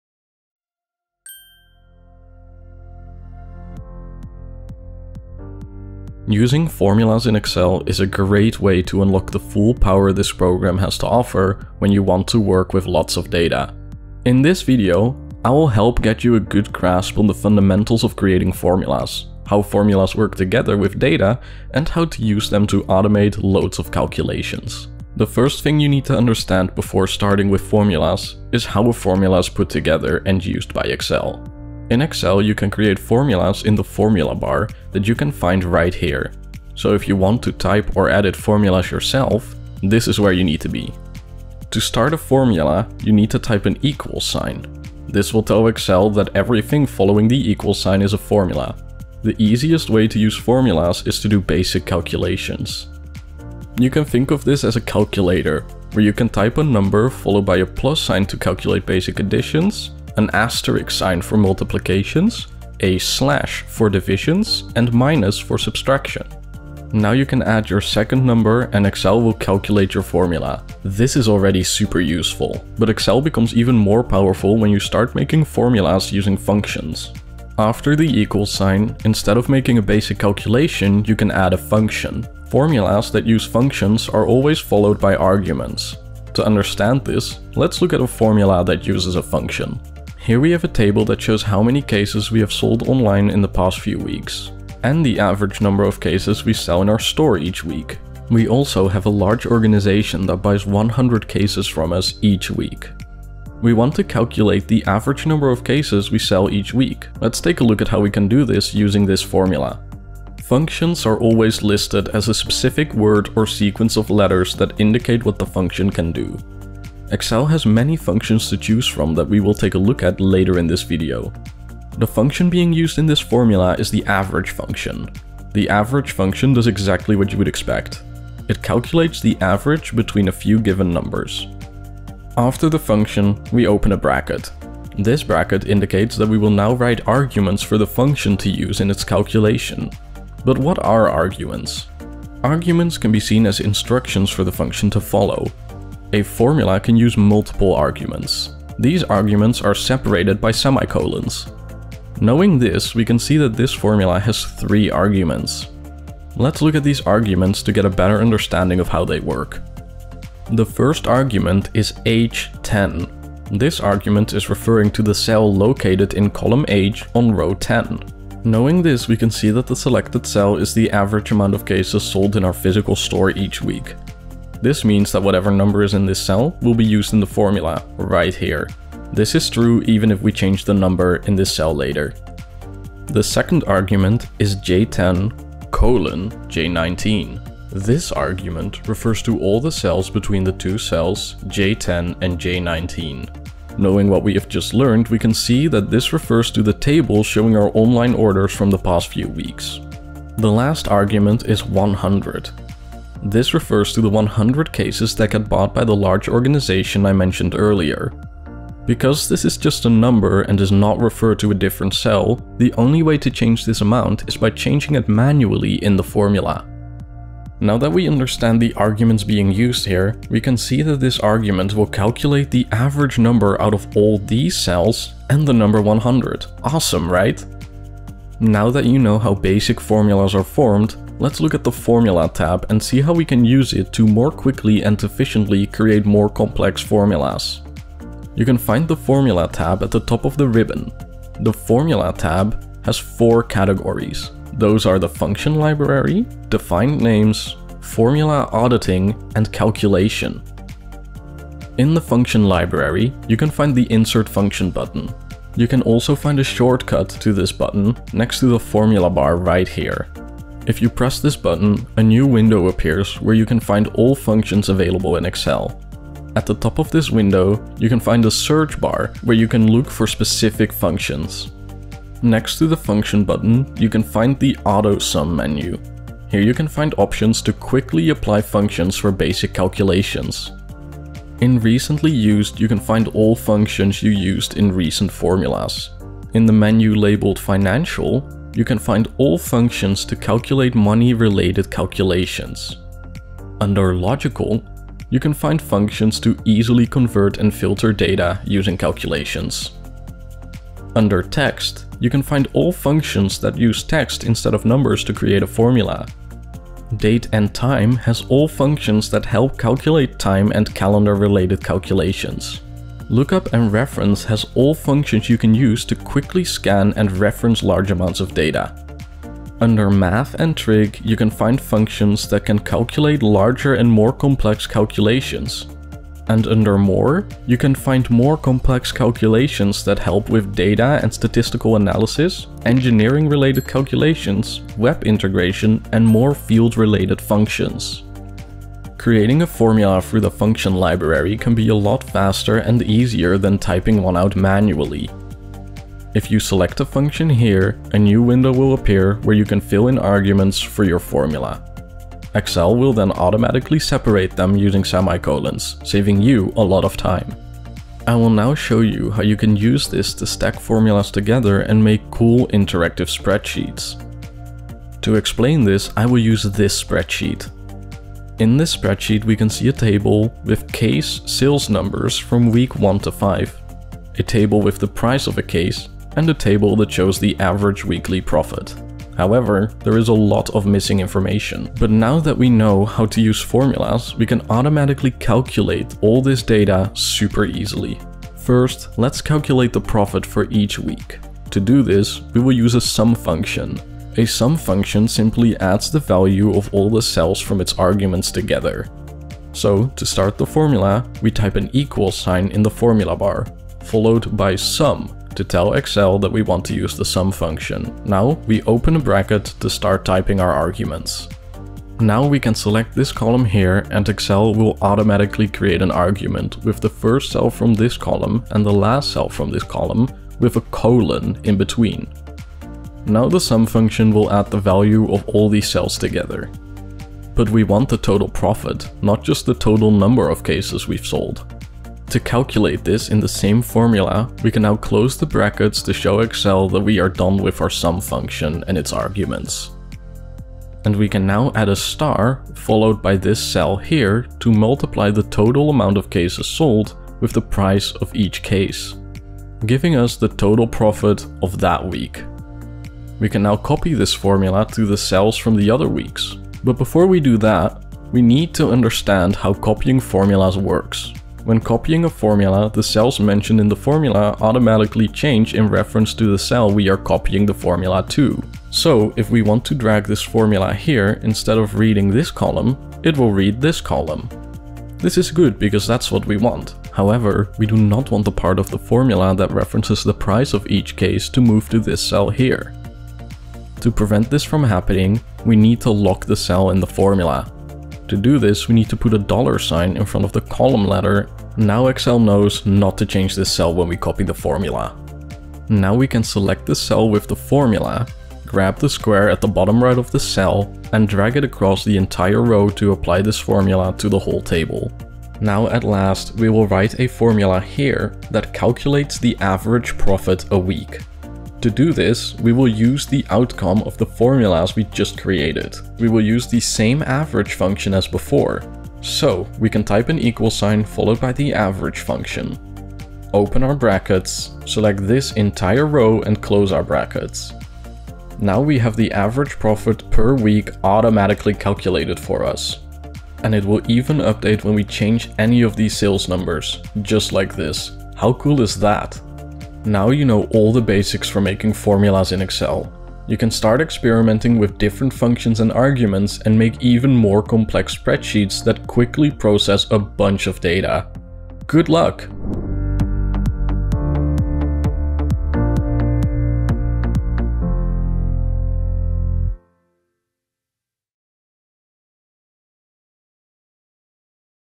Everything you need to know about formulas in Excel!
Excel has many formulas that you can use to really enhance your spreadsheets and make advanced calculations. In this video we go over a bunch of these formulas so you can start using them effectively in your spreadsheets!

Host: Pepijn van Setten
Writer/editor: Pepijn van Setten